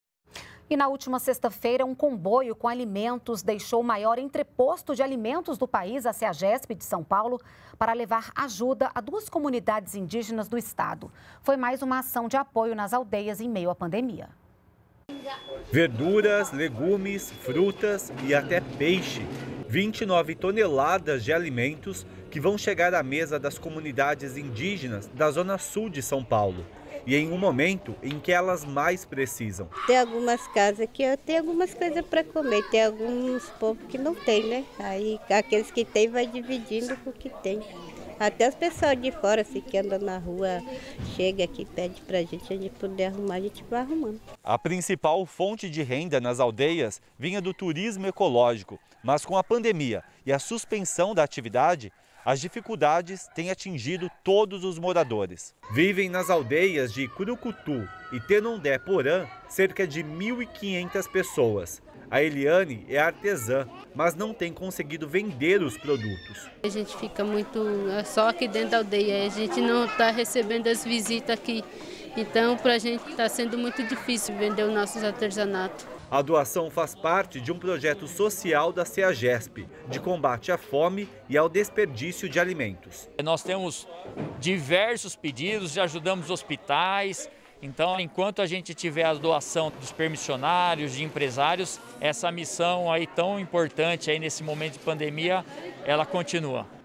Indígenas recebem verduras, frutas e peixes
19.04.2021 - Na última sexta-feira, um comboio com mantimentos deixou o maior entreposto de alimentos do país, a Ceagesp, em São Paulo, para levar ajuda a duas comunidades indígenas do estado. Foi mais uma ação de apoio nas aldeias em meio à pandemia.